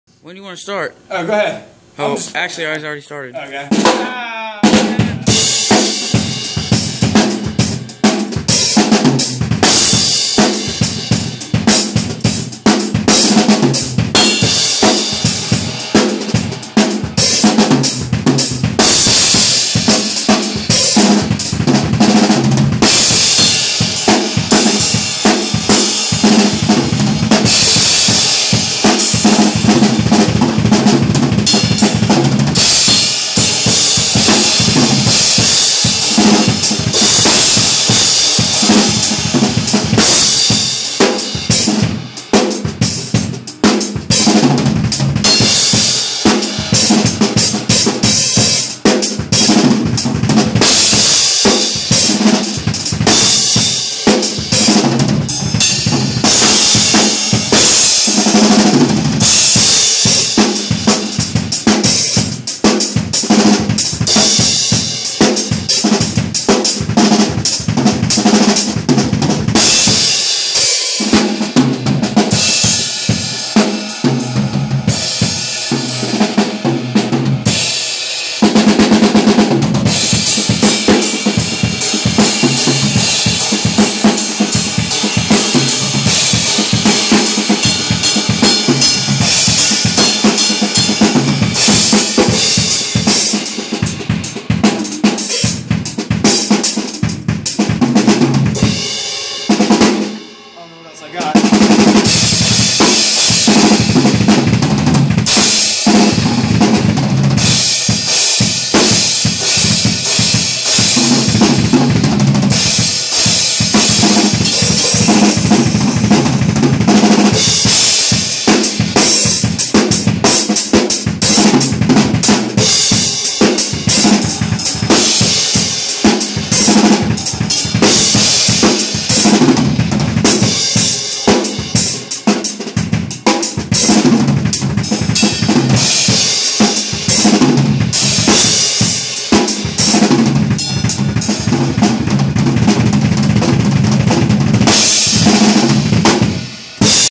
Drum solo at band practice
My guitar player Mark Tisdel filmed me doing a lil improv/warm-up at Last One There practice.
I play:
Imperial Cymbals & Lowboy Beaters
Check us out!